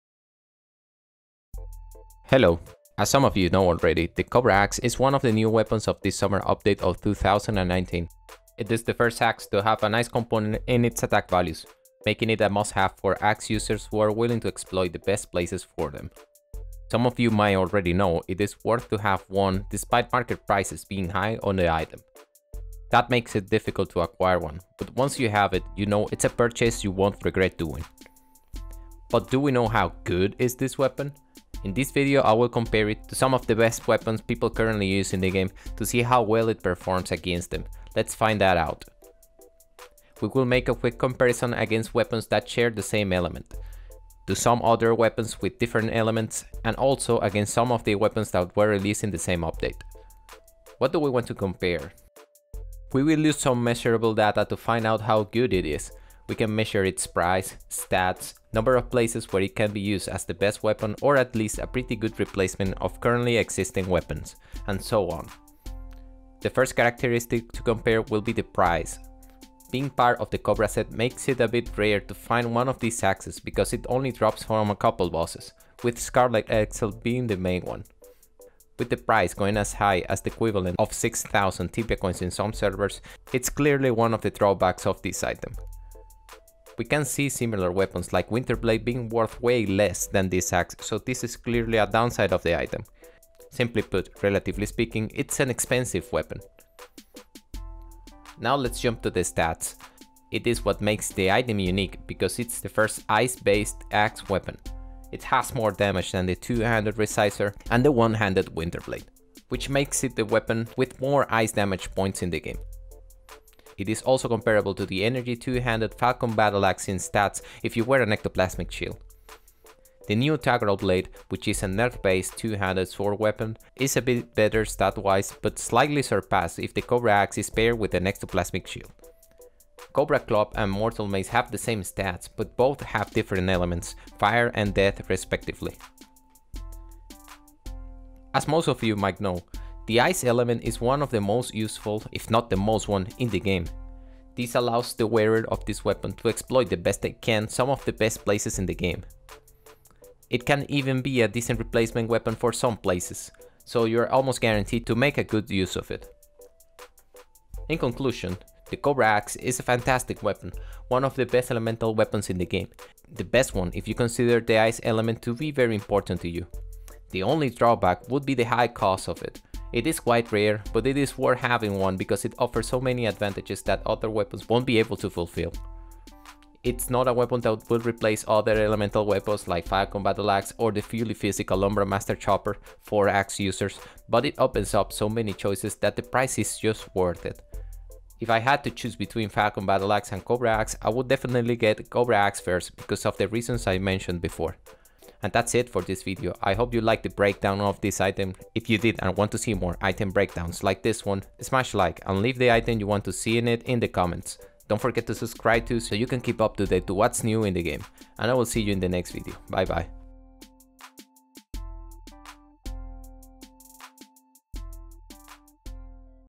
COBRA AXE: How GOOD is it?
This is a comparison video of the Cobra Axe, check it out!